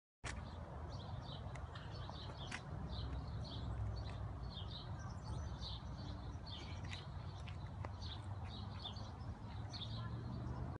OKC Fan Burns Durant Jersey On Twitter
After Monday's announcement Kevin Durant is heading to the Warriors, an angry Oklahoma City fan tweeted video of himself burning the MVP's jersey.